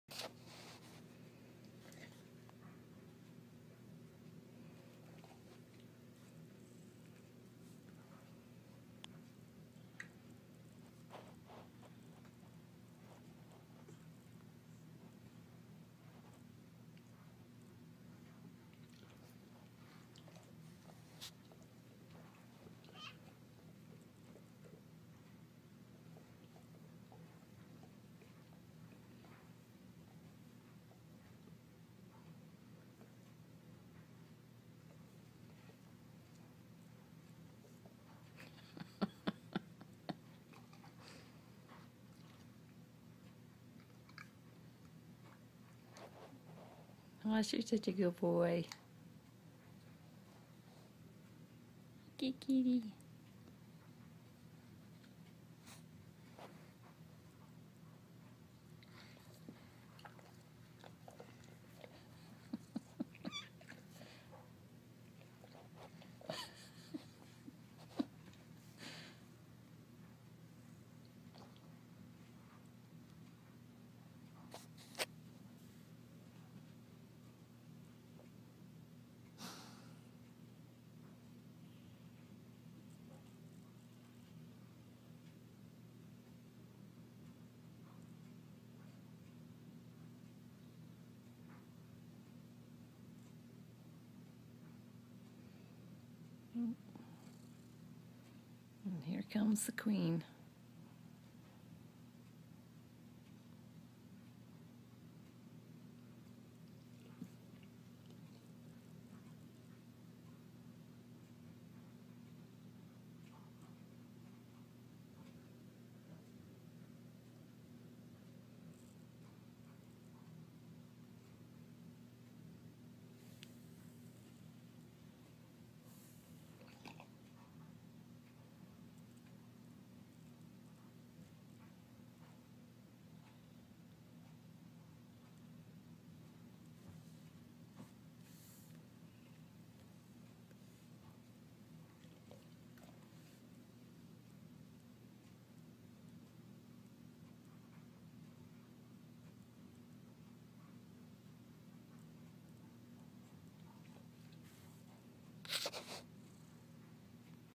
Orphaned kitten were found in a barn last week.German Shepherd has become a surrogate mom
Orphaned kittenwere found in a barn last week.German Shepherd has become a surrogate mom